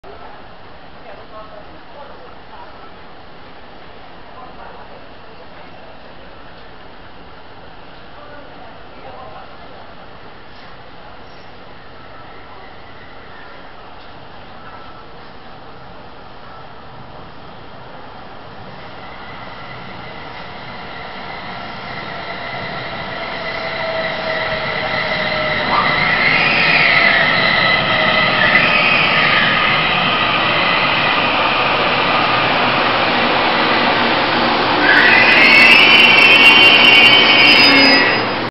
台鐵1201次EMU800型區間車進站(松山)
台鐵1201次EMU800型區間車進站(松山),攝於2014年1月16日,去拍捷運囉~